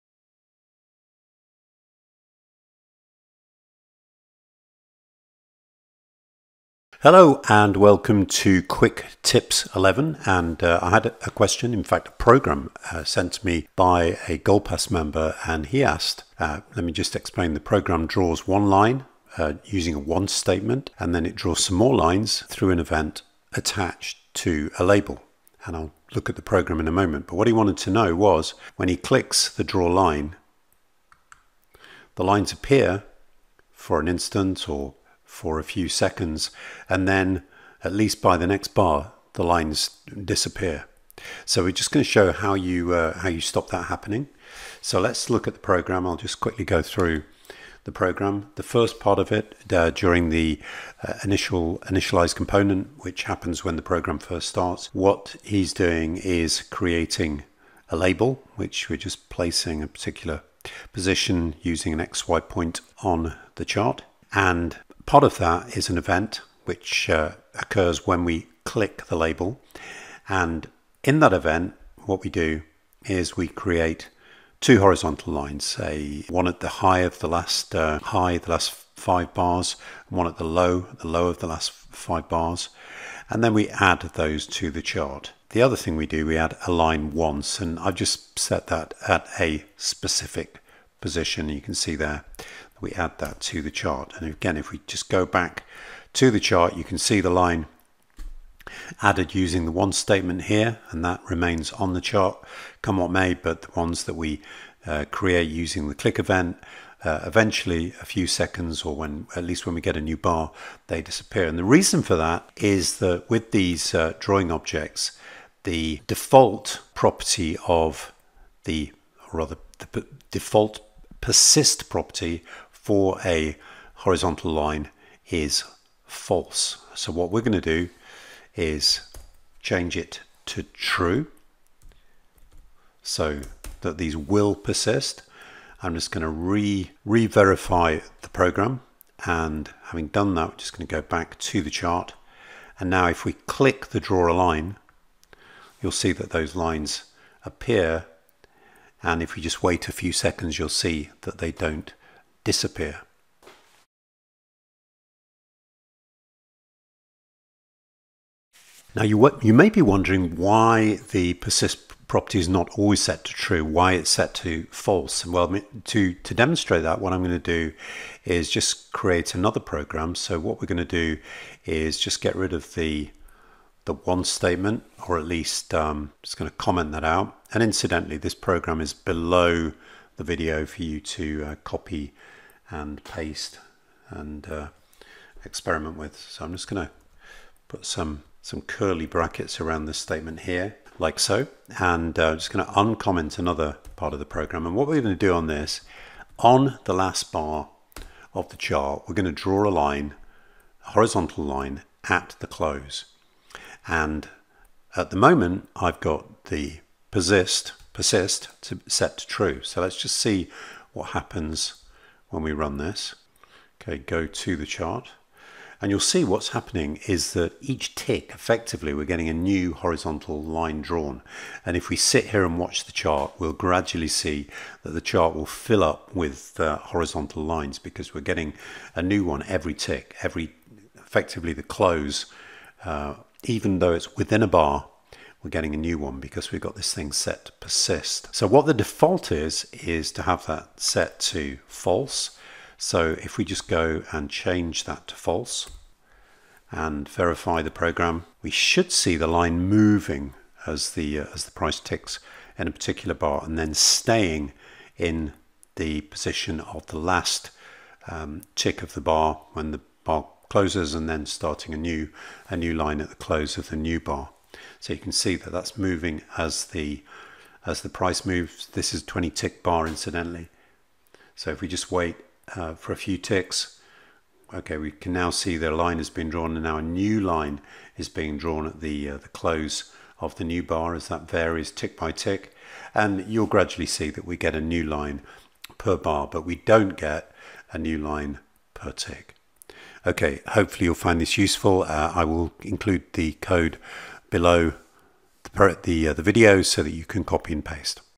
Quicktips 11 | Using the persist property with drawing objects
In Quicktips 11 I discuss a program sent to me by a Gold Pass member. In the program a text label is created with a click event that draws two horizontal lines. After a few seconds the horizontal lines disappear. The quick tips explains how this behavior can be controlled using the persist property of the horizontal line object.